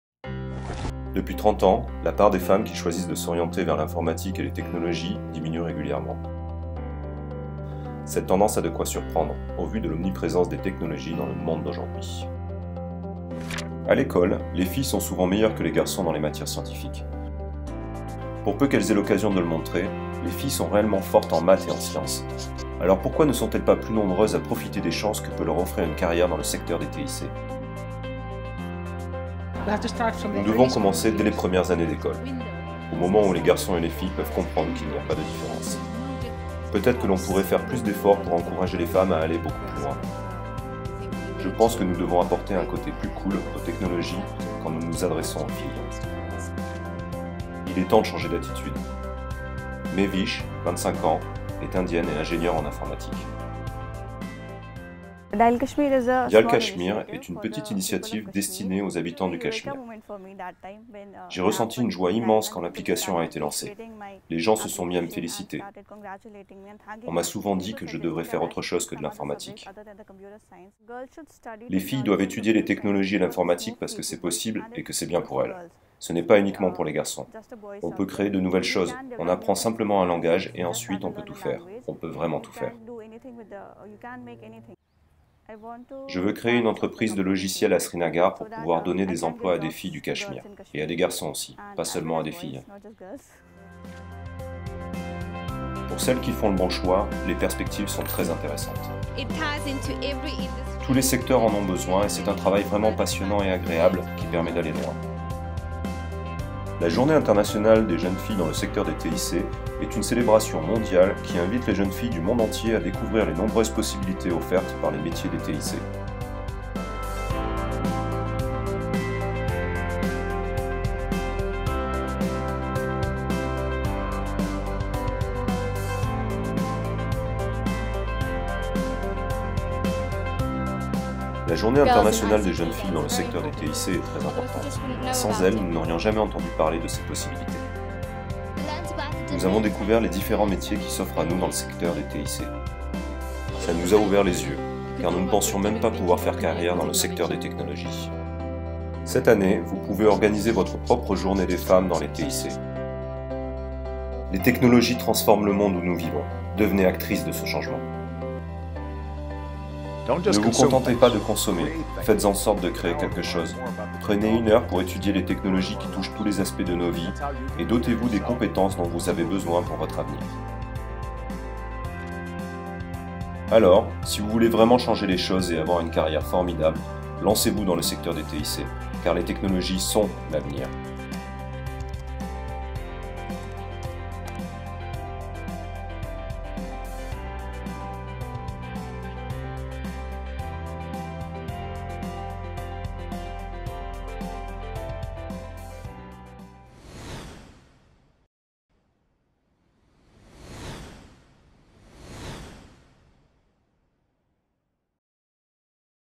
International Girls in ICT Day 2015 Promotional Video French
​​​​International Girls in ICT Day is celebrated annually around the world on the fourth Thursday in April.

It gives girls and young women the opportunity to engage with information and communication technologies (ICTs) in a range of exciting ways, encouraging them to consider a future in the technology sector. To date, over 111,000 girls and young women have taken part in more than 3,500 events held in 140 countries around the world.

Special Event at ITU, 23 April 2015
This year, Girls in ICT Day is celebrated on 23 April, 2015. ITU will host a special event in ICT Discovery, our museum dedicated to the history of telecommunications at ITU Headquarters in Geneva. Participants are invited to attend workshops on satellites, creative coding, mobile apps and robotics. This will be followed by a Career Fair, where attendees will have a chance to meet female ICT professionals and learn about relevant study programmes and job opportunities in the sector.